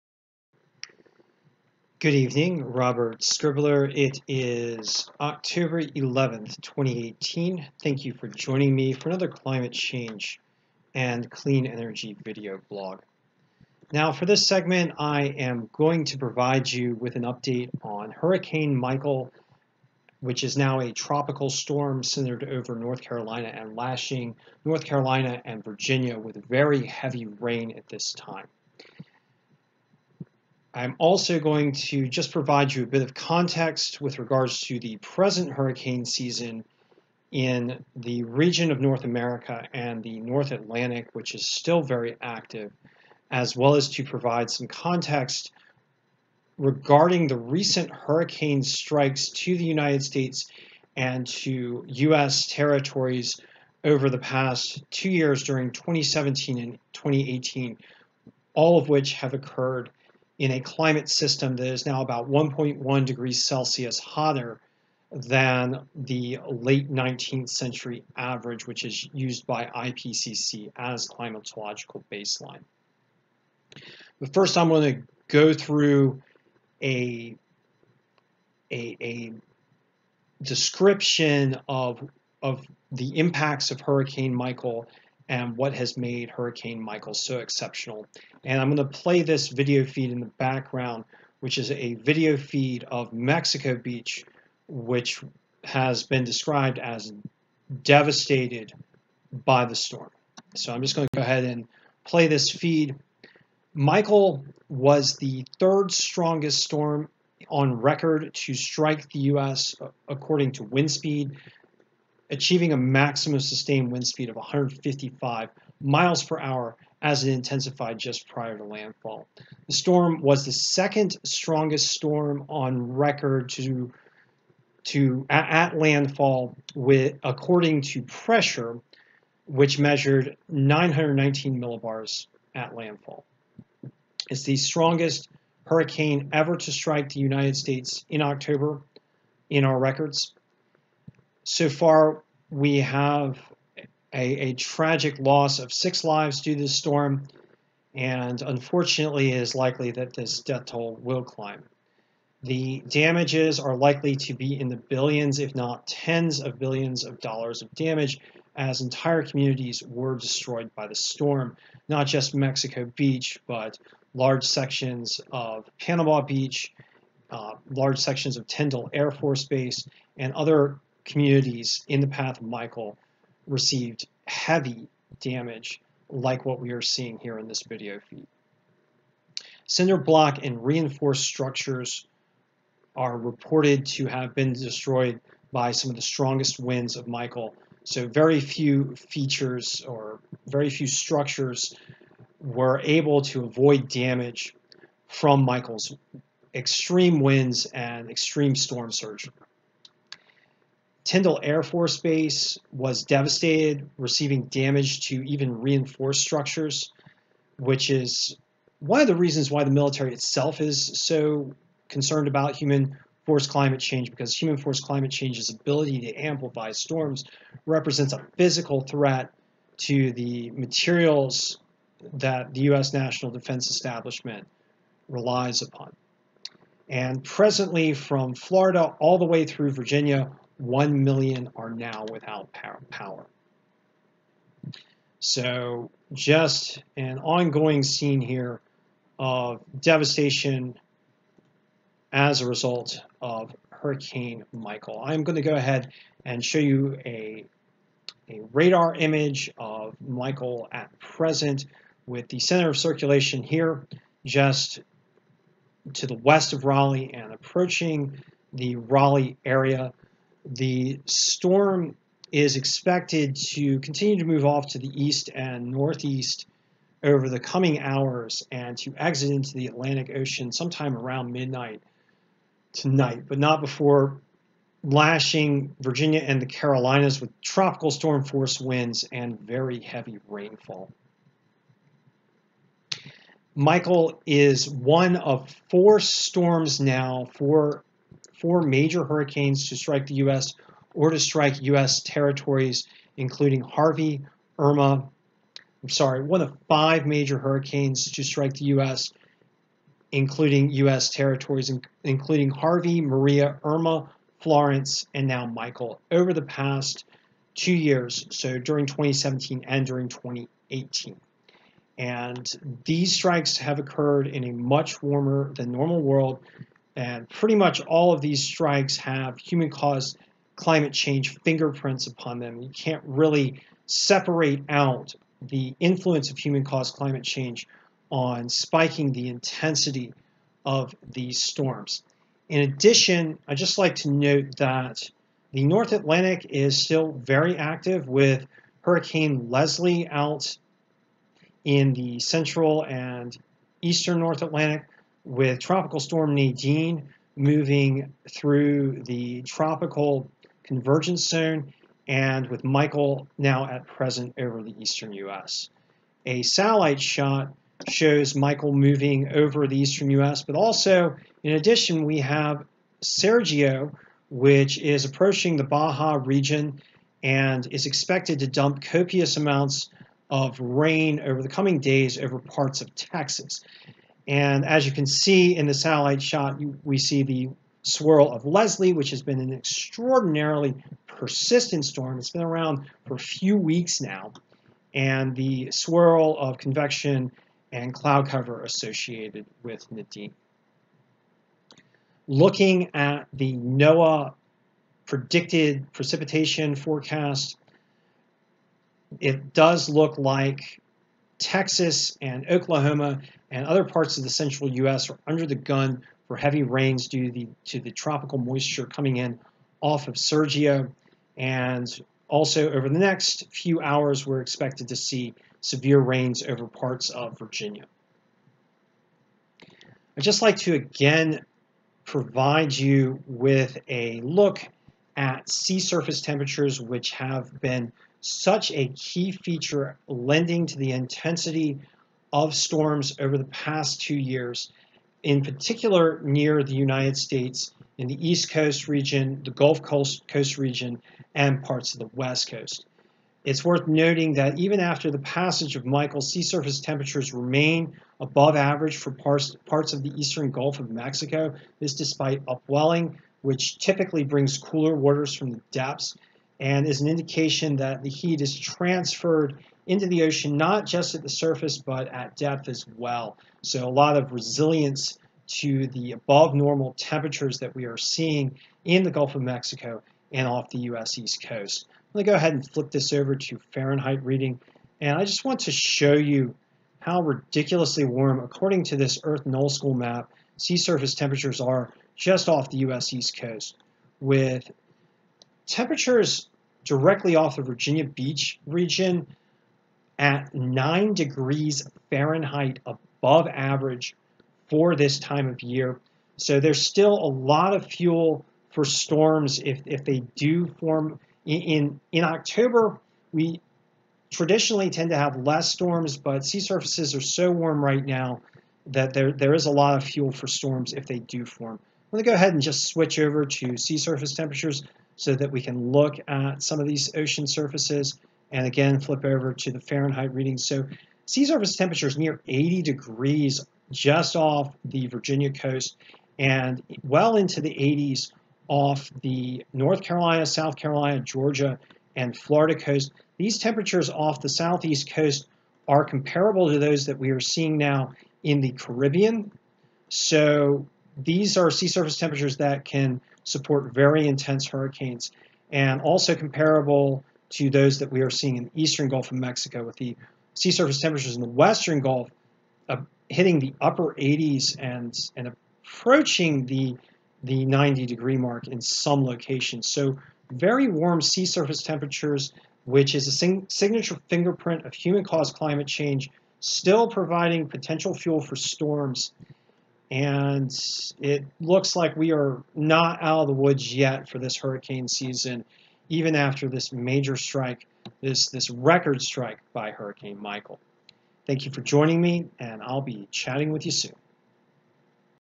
Michael Produces Swath of Destruction
Hurricane Michael was the most powerful storm to strike the U.S. in the month of October. The storm has devastated numerous communities along the Gulf Coast, resulted in the tragic loss of at least six souls, knocked out power for about a million customers, and likely resulted in damages in the billions of dollars. The storm is now one of five major hurricanes to impact the U.S. and its territories since 2017 -- a time when global temperatures have risen to around 1.1 C above late 19th Century averages.